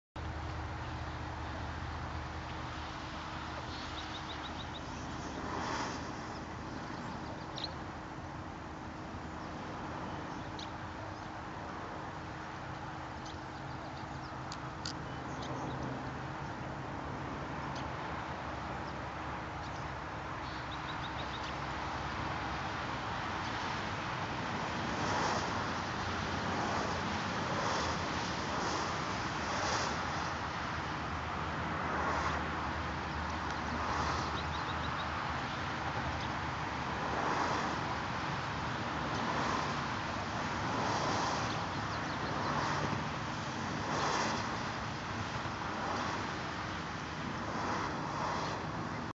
California Quail Walking and Feeding
The California Quail is our state bird.  California quail are so beautifully appointed - and such fun to watch.  They also have about a 30-foot flush distance, so it can be difficult to see them up close.